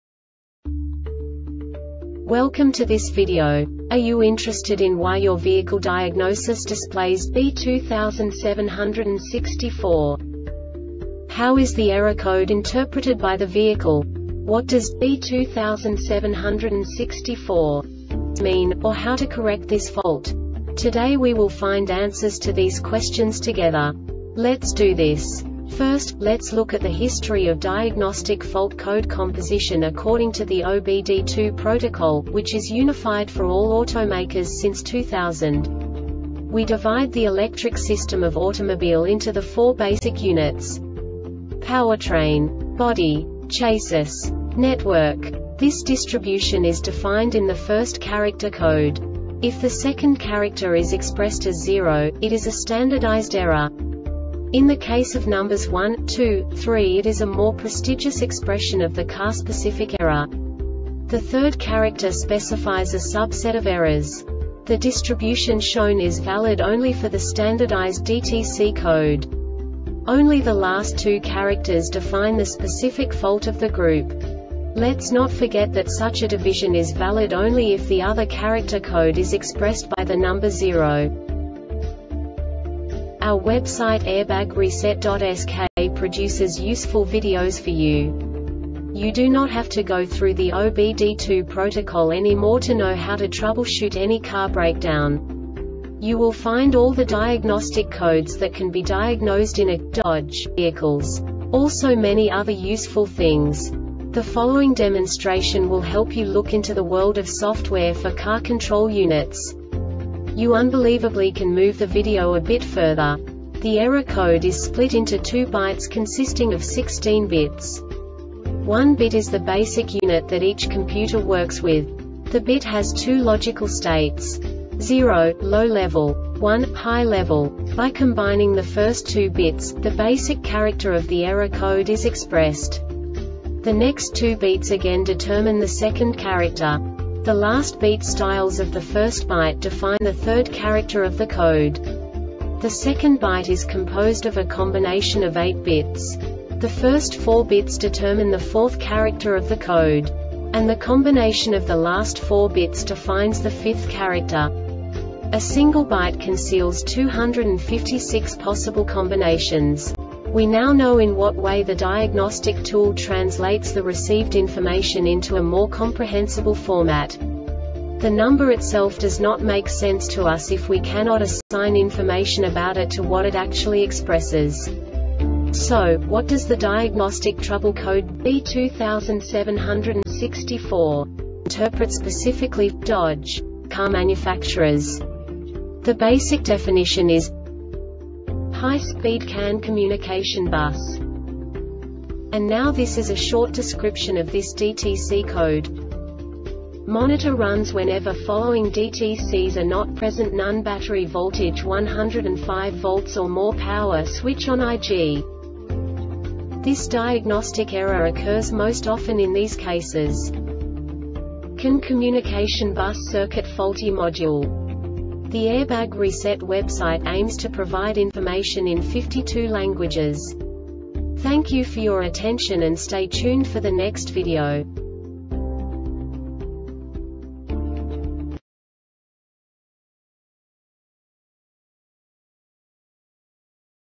DTC Dodge B2764 Short Explanation
Contents:
0:21 Basic DTC analysis according to OBD2 protocol standard.
1:48 Insight into programming
2:58 A diagnostically understandable description of the Diagnostic Trouble Code
3:05 Basic error definition

Make:
Dodge
Code:
B2764
Definition:
High Speed CAN Communication Bus
Description: